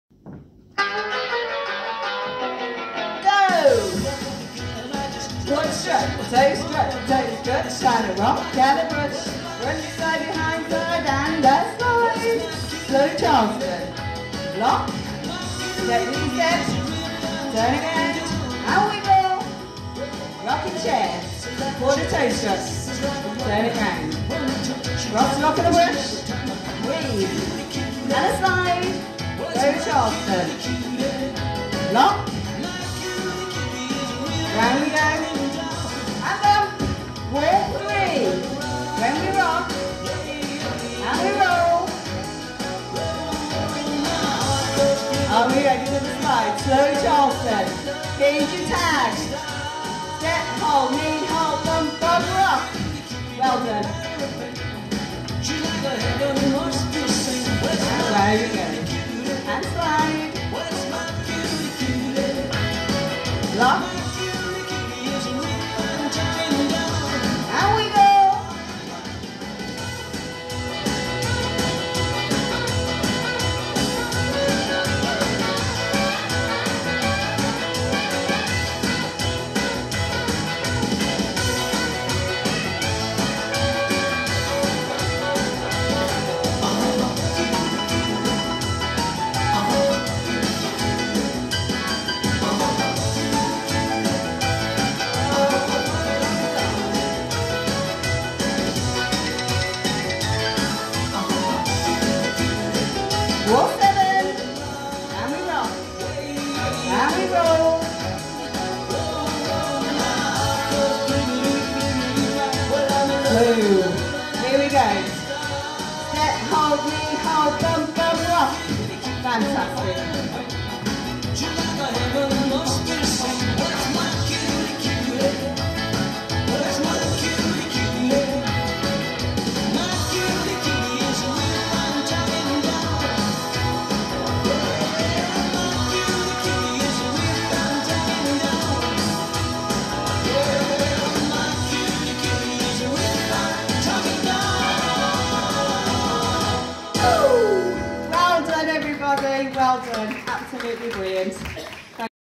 My Cutie line dance
48 count, 4 wall improver line dance with 2 tags, choreographed by Vikki Morris